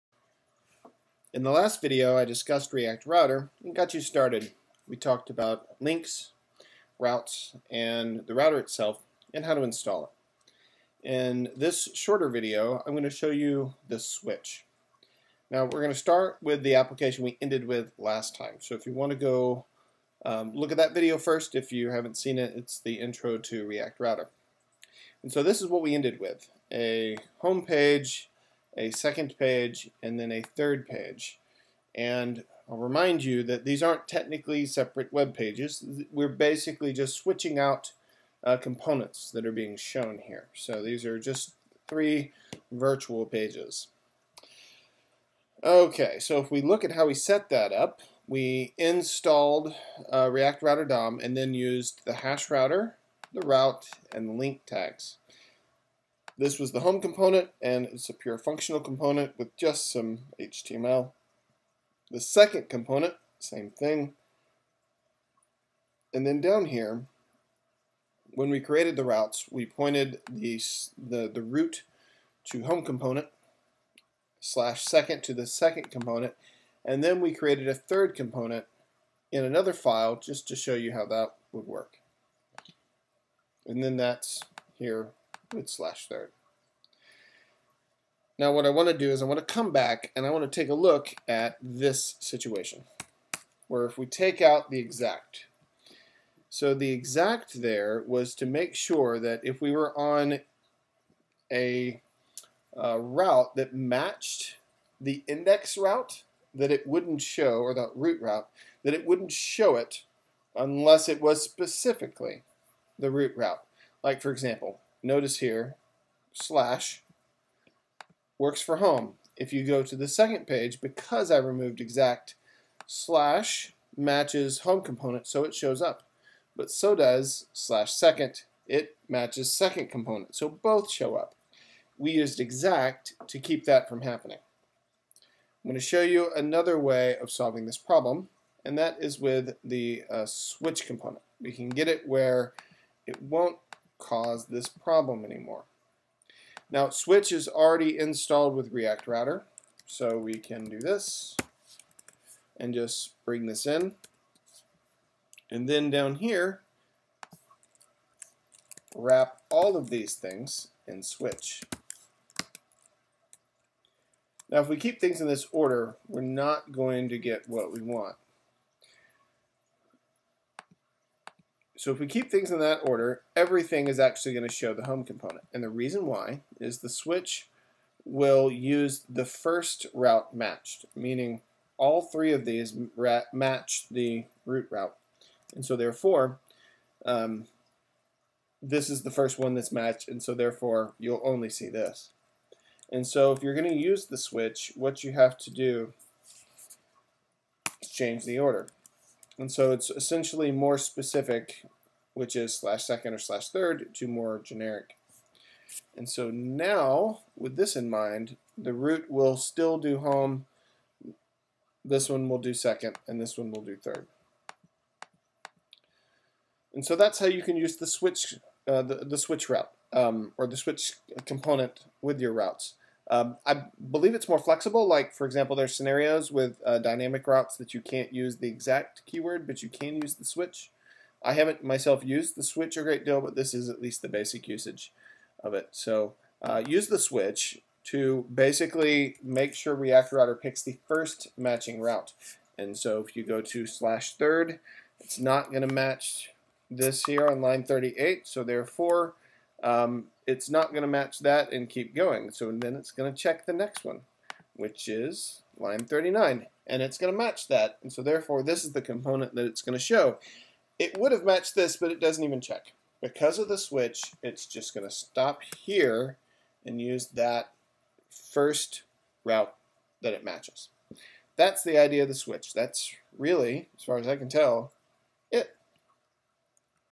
React Router: Switch - React Tidbits
In this video I show you how to use the Switch component from ReactRouter.